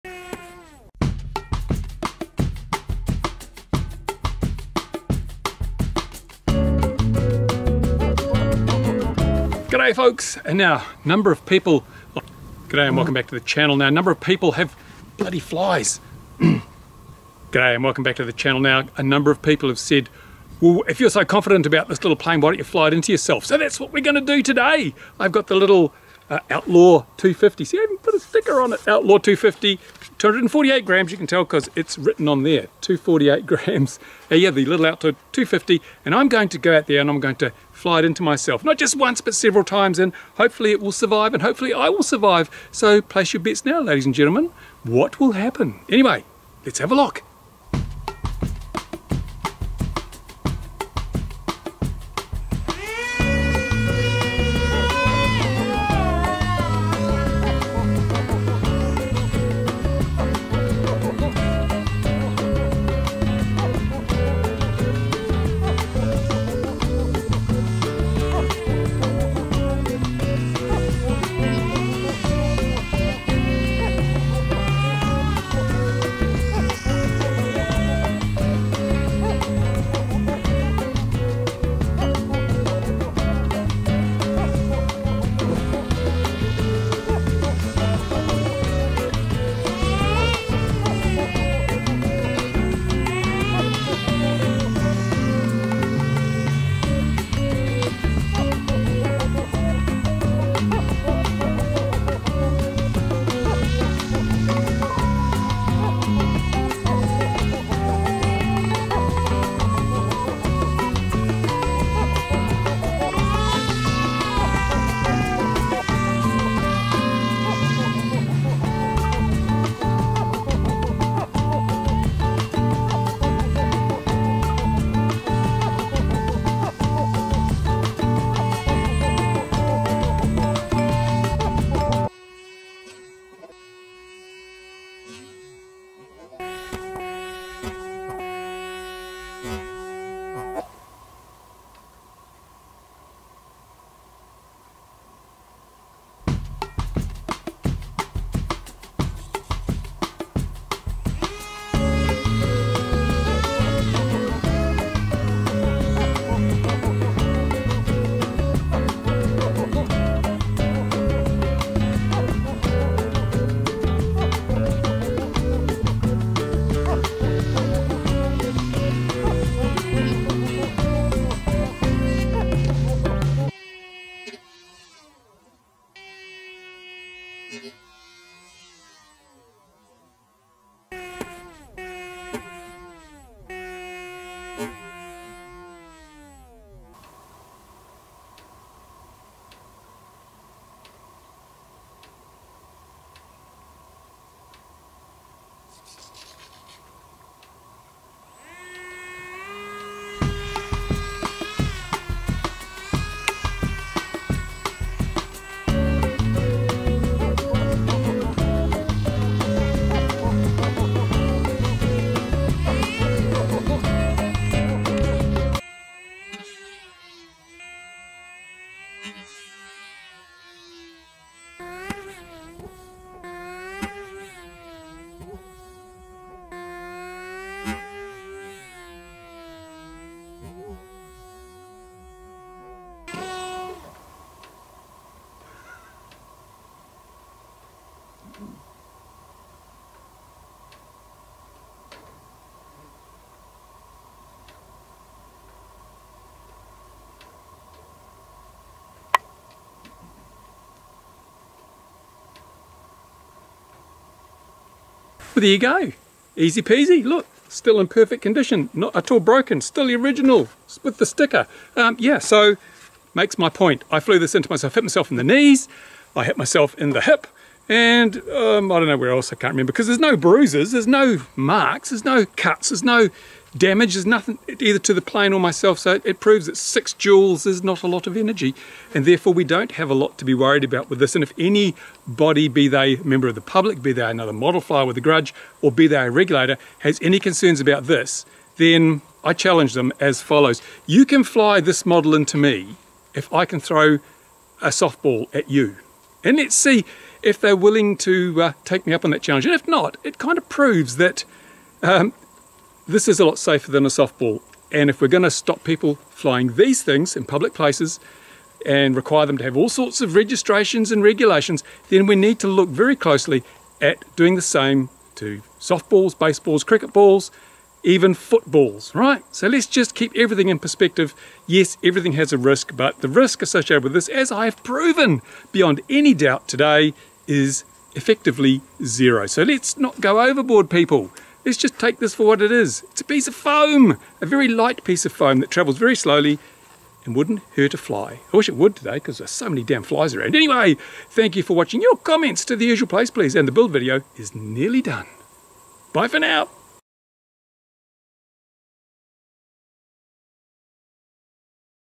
Will this hurt?
Viewers asked me to crash the Outlaw 250 FPV RC plane into myself to prove that it was as safe as I claimed... so here, by popular demand, is that video.